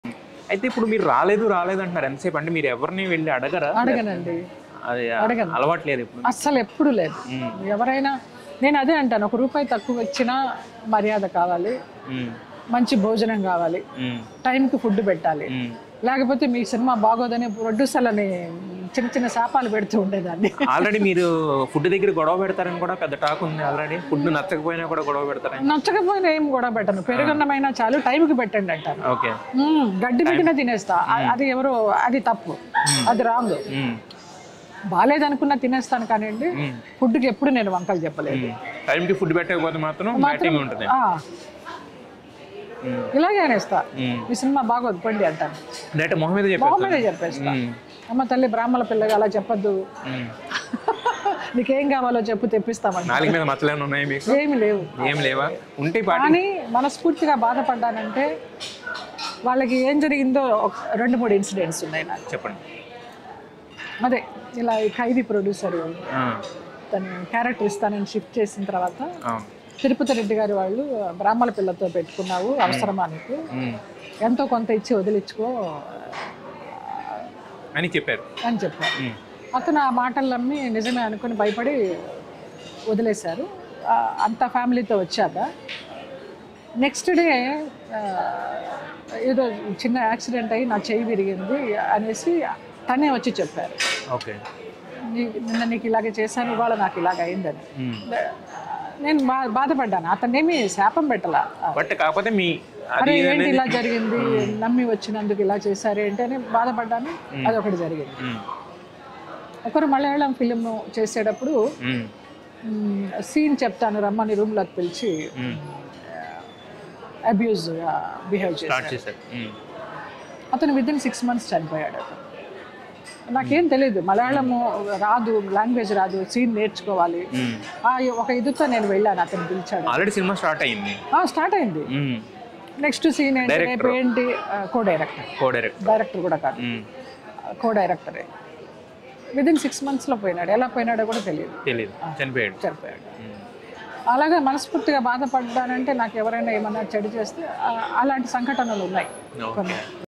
Jayalalitha Reveals Her Bad Experiences In Film Industry || Tarak Interviews
సీన్ చెబుతా అని రూమ్ లోకి పిలిచి...
ఆరునెల్లలో చచ్చాడు